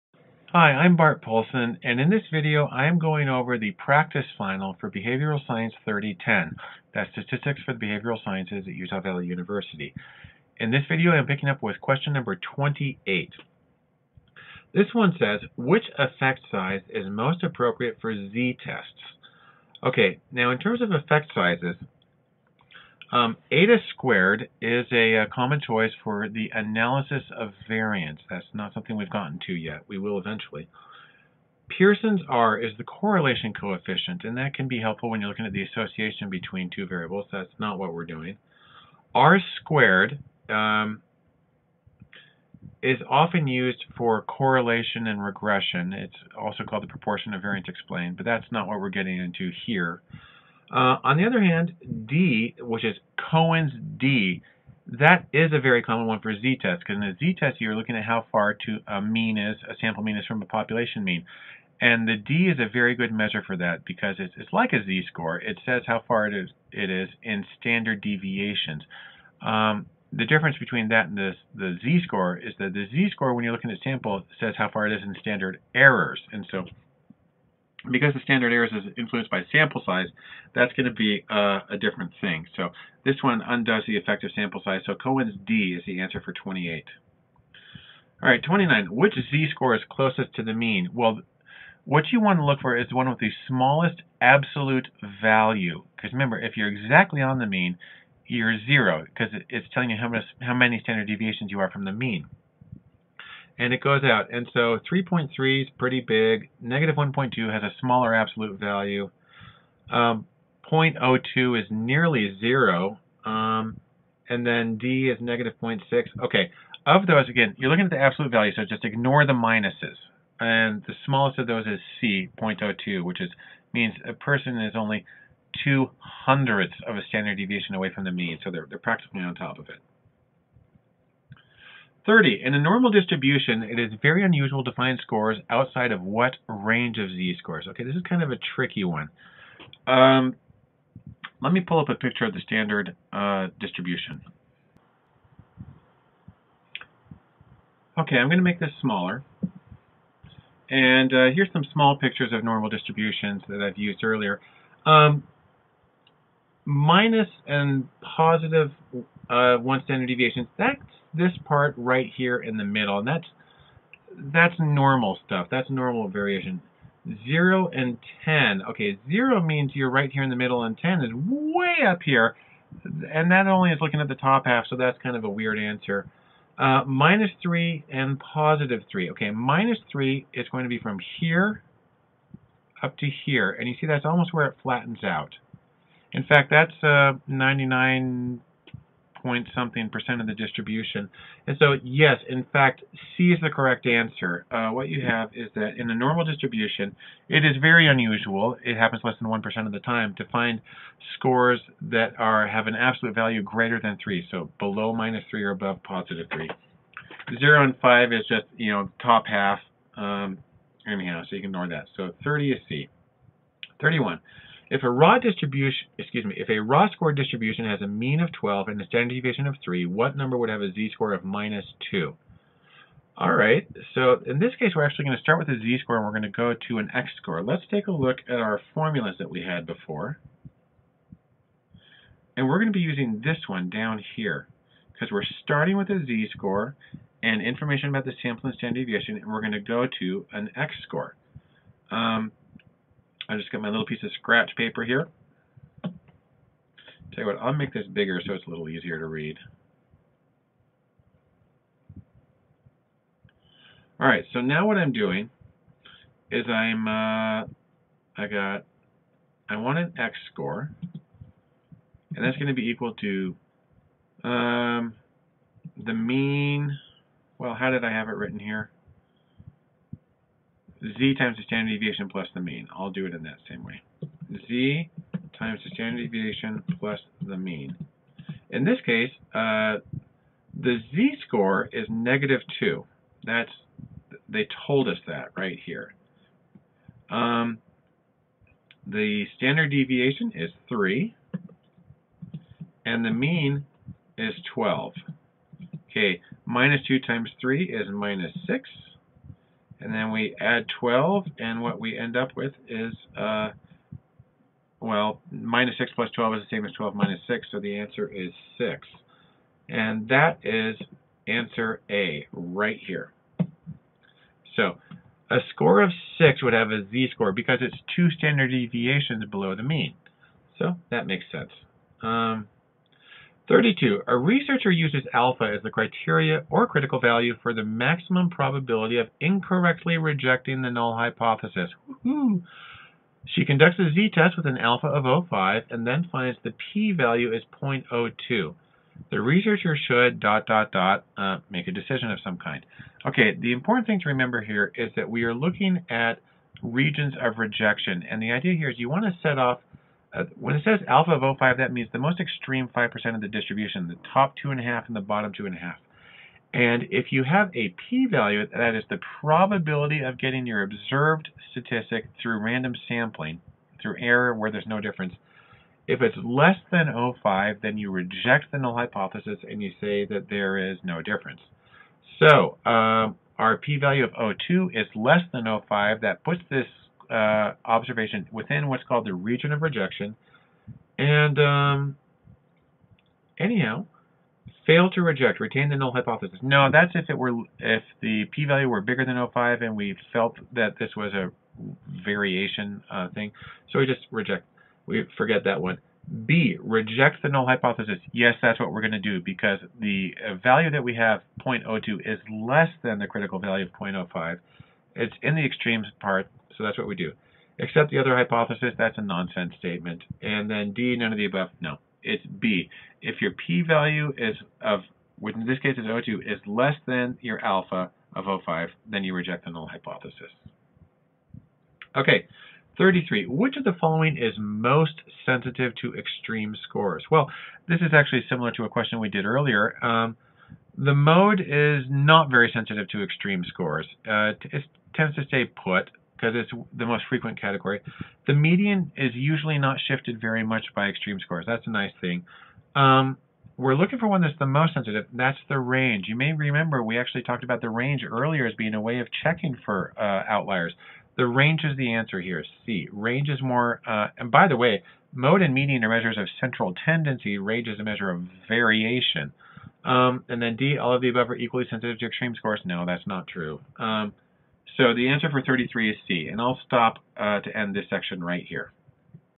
Statistics Practice Final - Part 4 (Q 28-33)
Review of questions on the practice final exam for BESC 3010: Statistics for the Behavioral Sciences at Utah Valley University.  This is based on "BESC 3010 Practice Final.pdf," which is available in Canvas under Pages/Practice Final or at j.mp/besc3010final